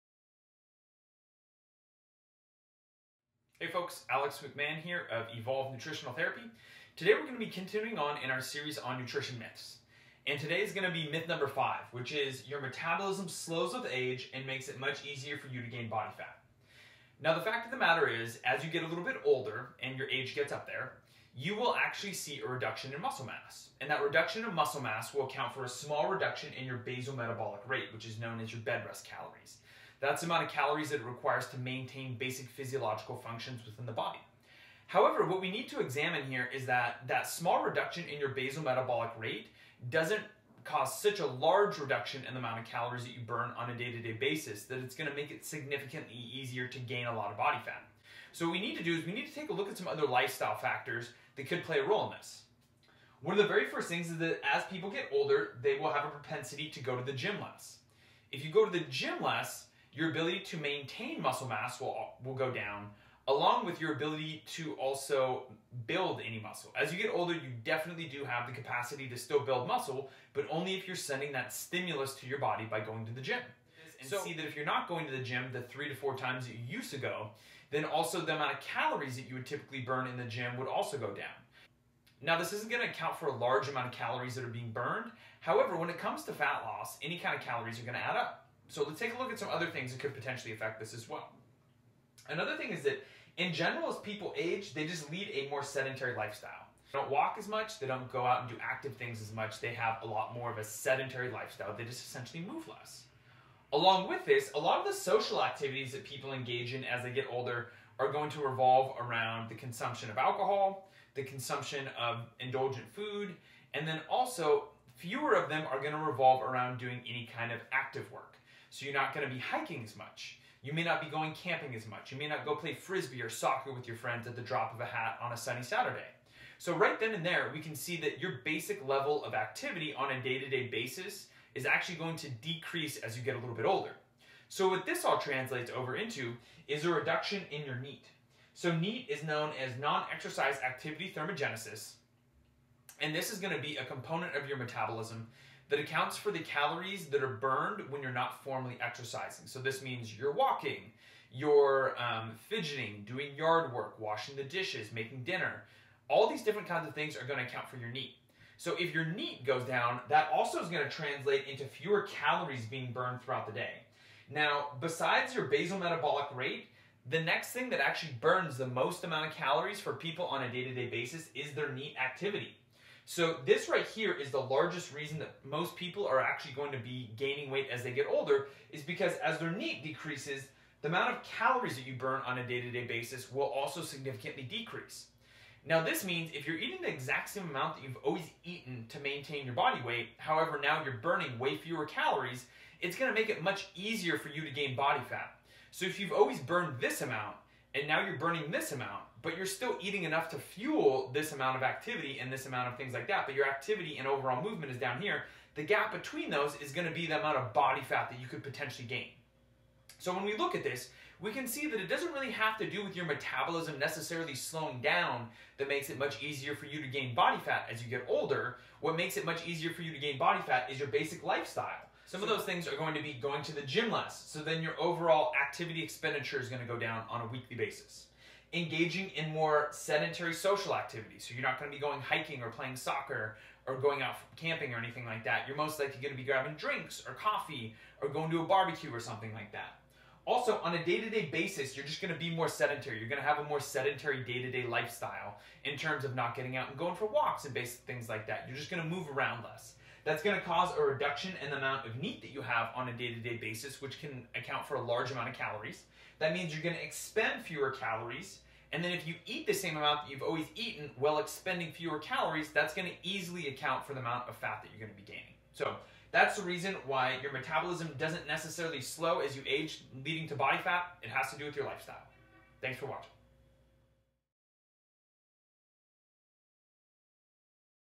Nutrition Myth #5: Your Metabolism Slows Down As You Age
It's common to hear someone say that they can't eat how they used to or else they'll gain a ton of weight. They'll chalk it up to their metabolism slowing down, but is this true ? There's components of older individuals lives that do make them burn fewer calories, click play to find out what it is!